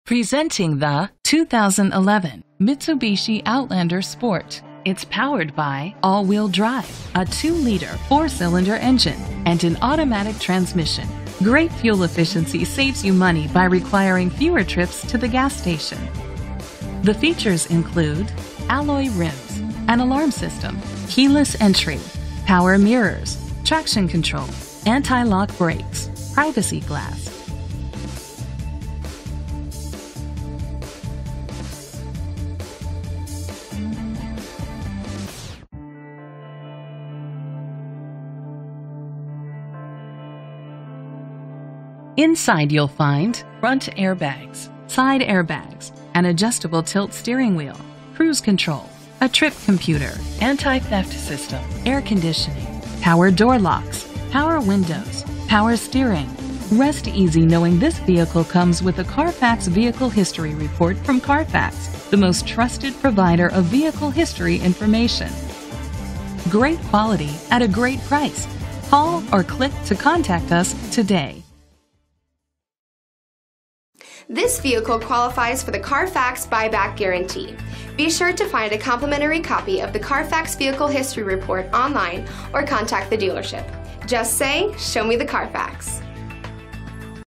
2011 Mitsubishi Outlander Sport 12730 - Turnersville NJ
Phone:
Year: 2011
Make: Mitsubishi
Model: Outlander Sport
Trim: SE
Engine: 2.0 liter 4 cylinder AWD
Transmission: Automatic
Color: White
Mileage: 77701
Address:
VIN:JA4AR4AU2BZ012730